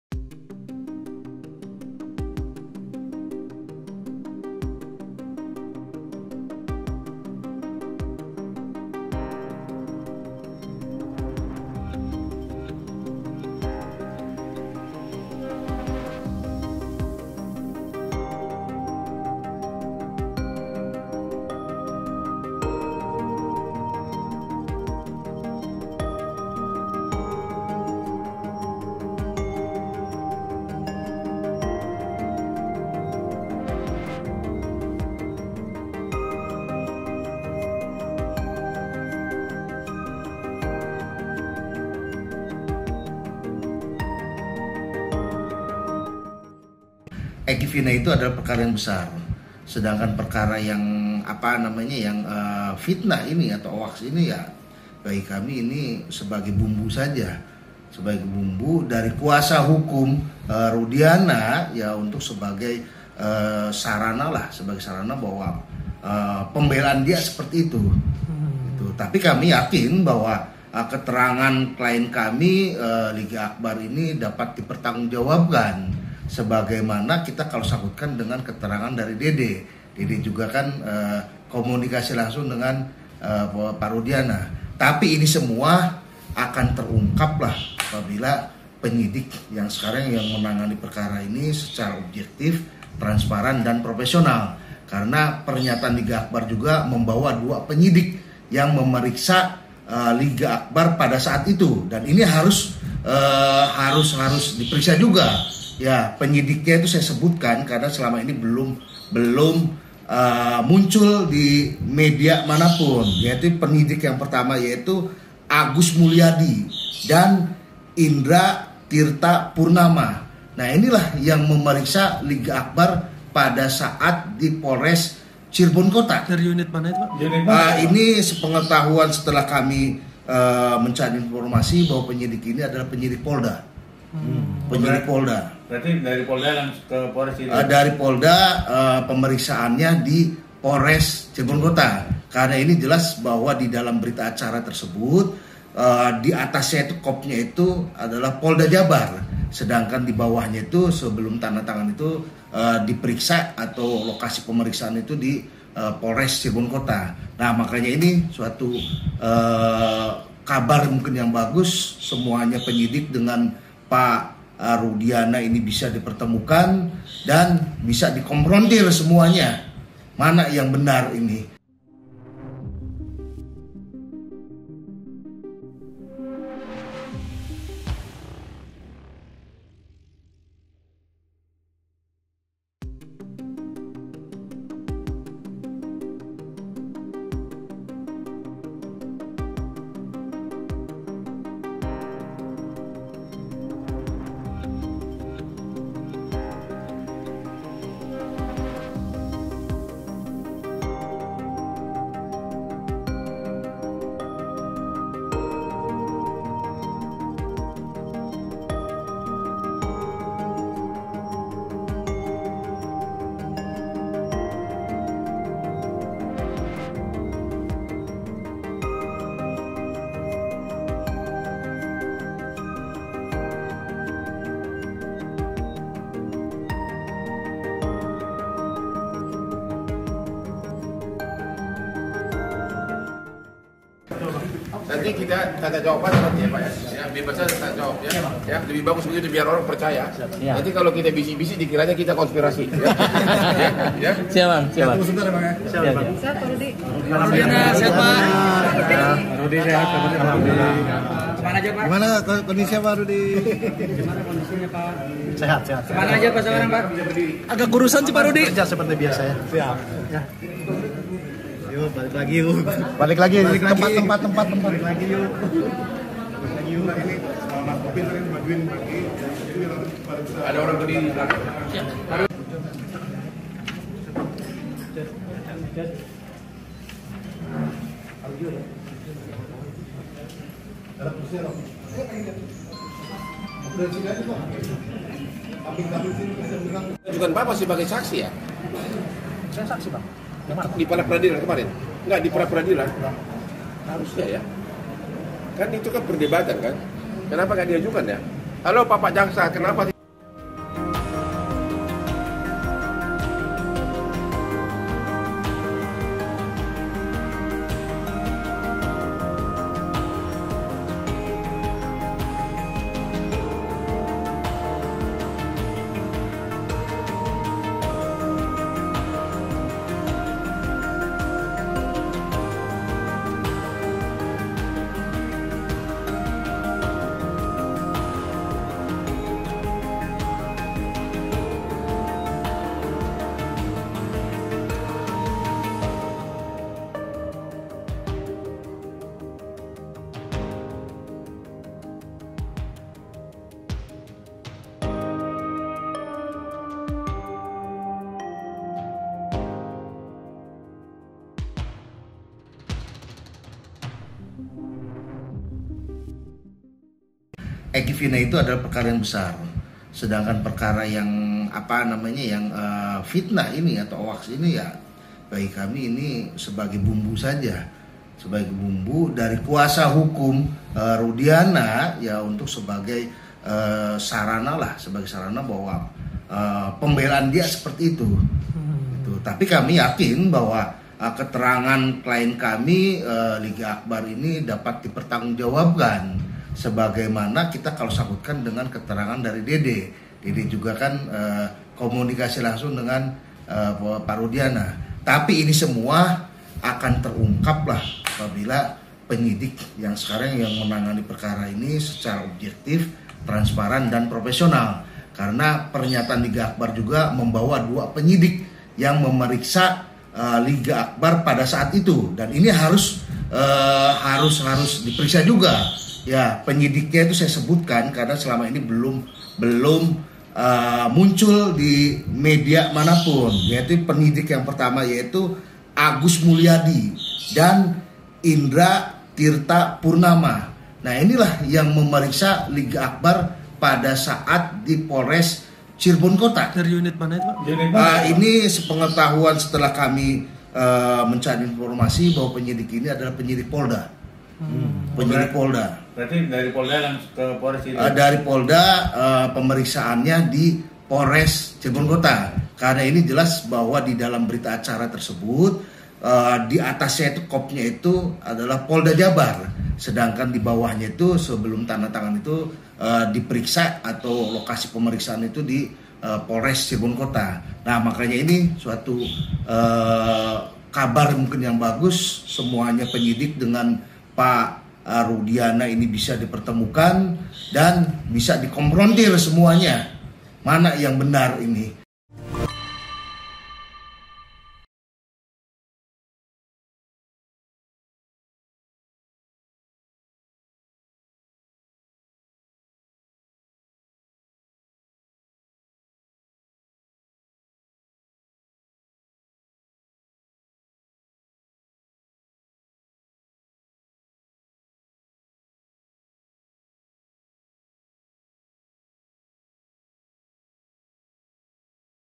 Liga Akbar Siap Dikonfrontasi dengan Iptu Rudiana soal Kesaksiannya dalam Kasus Vina dan Eky
Liga Akbar yang telah mencabut keterangan BAP pada 2016 silam dalam kasus pembunuhan Vina dan Eky mengaku siap menghadapi laporan yang dilayangkan oleh pihak Iptu Rudiana.

Dia juga tak khawatir jika harus dikonfrontir dengan Rudiana untuk membuktikan pihak mana yang berbohong. Dia juga berharap agar penyidik yang memeriksanya saat itu dan disebut melakukan tekanan/penganiayaan juga diperiksa.

Pernyataan tersebut disampaikan kuasa hukumnya, Yudia Alamsyah. Menurut Yudi, kliennya akan bersikap kooperatif dalam proses penyelidikan.

Sebelumnya, Liga Akbar telah mencabut keterangannya di BAP 2016 lalu. Dia menjelaskan, pengakuannya kala itu karena ada tekanan dari penyidik untuk menambahkan keterangan kronologis agar Liga Akbar mengakui berada di TKP serta mengetahui adanya pelemparan dan pengejaran. Namun, keterangan tersebut telah dicabut oleh Liga Akbar.